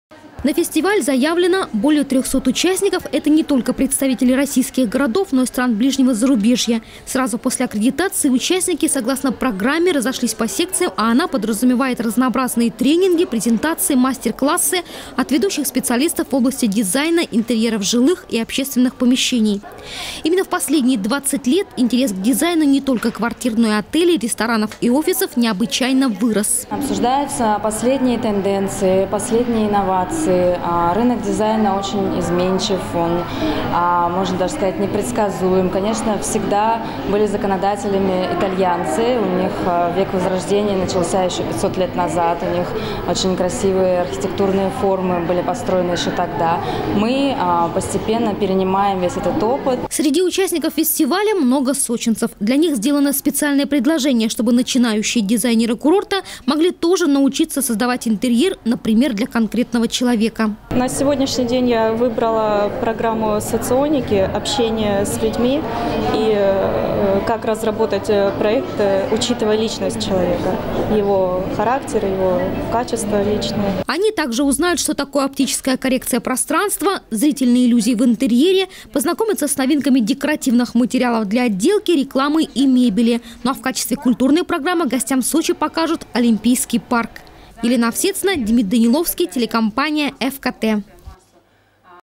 Sochi Design Fest 2014 открылся в Сочи. Новости 24 Сочи Эфкате
Это профессиональный форум для российских архитекторов, дизайнеров, художников представителей мебельных компаний и специалистов свето-технического оборудования.
Сочи. Новости.
Телеканал Эфкате.